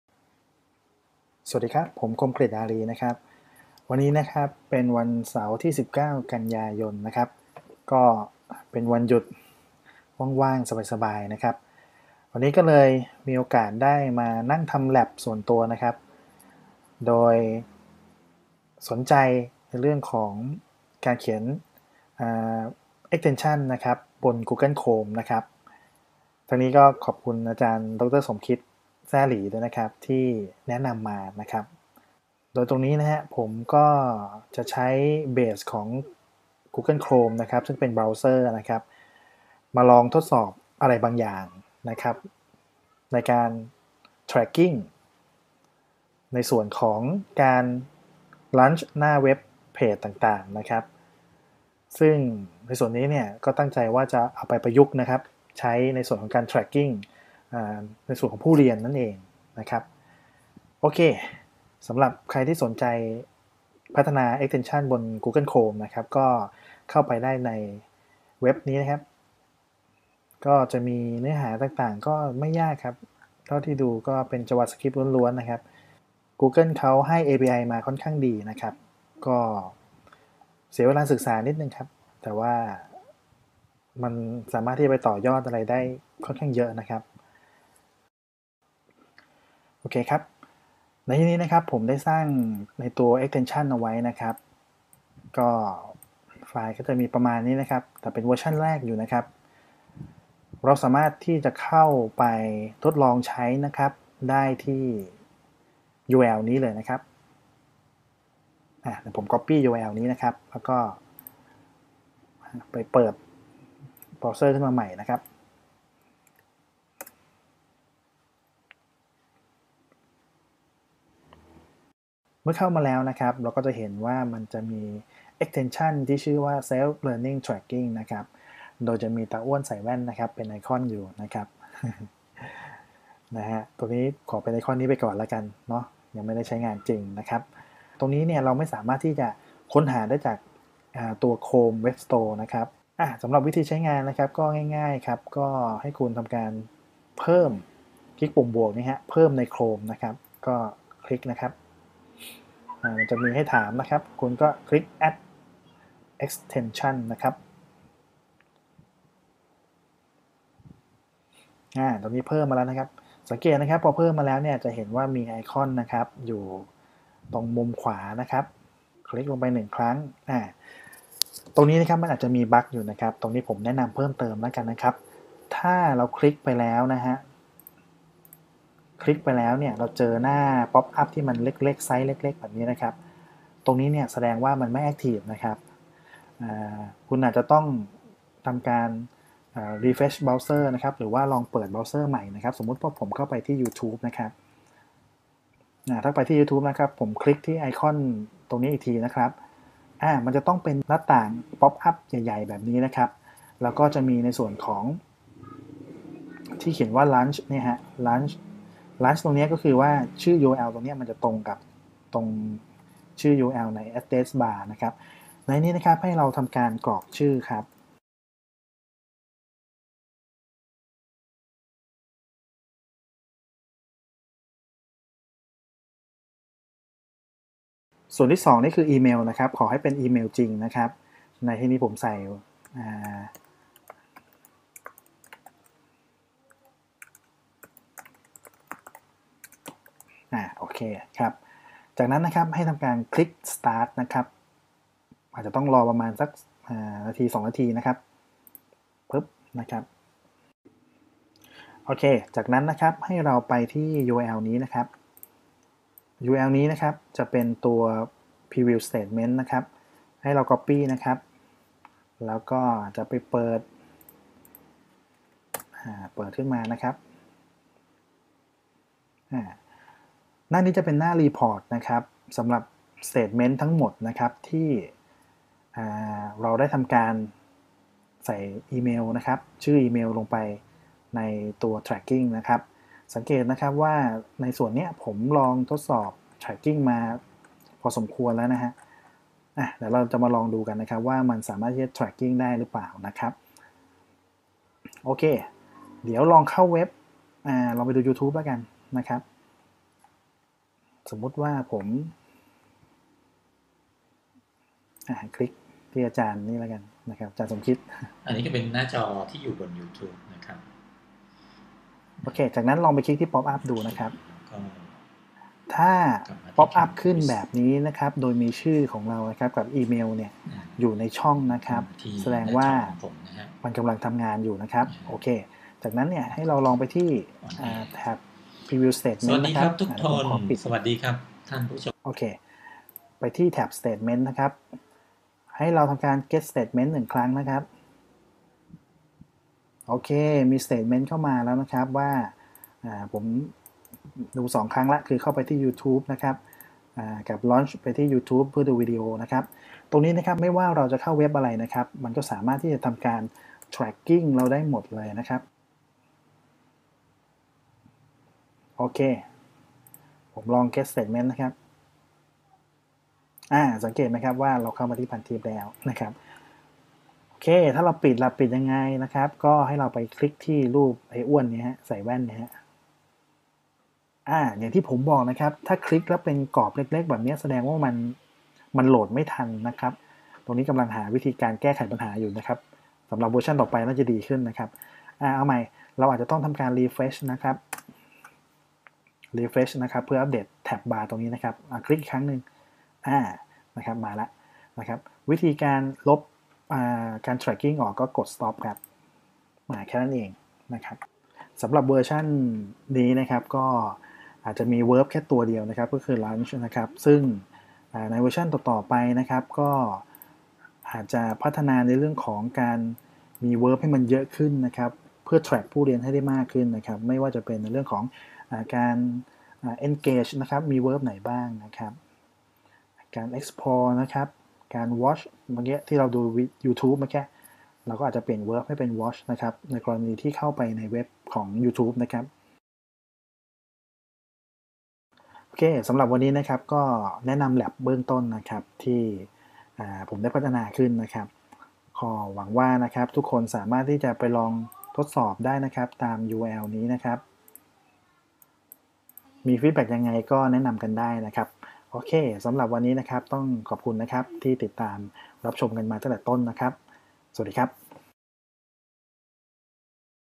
แนะนำ Self-Learning Tracking for Chrome Extension
แนะนำระบบ Self-Learning Tracking for Google Chrome Extension (version v1.0.0)

** ขออภัย ได้ปิดทำการทดสอบระบบแล้ว **